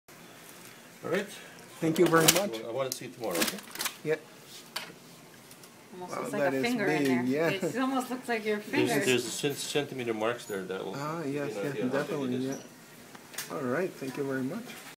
Ceci était dans la gorge a Castor !? 1/2
Je viens tout juste de me faire opérer dans la bouche, plus précisément sous la langue, un des mes conduit de salive sous la langue avait une pierre depuis plusieurs années...
Le chirurgien et l'aide infirmière ont été étonnes lorsqu'ils ont retirer ceci,
"that's so BIG !" elle a dit, le docteur a simplement lâcher un léger "wow" et un rire en douceur... pour ne pas me faire paniquer surement ehehehehe.
Ils n'ont probablement jamais vu une pierre aussi grosse, ils ont appeler les autres docteurs/infirmières de l'étage pour venir jeter un coup d'oeil.. ils voulais prendre des photos aussi haahahaha.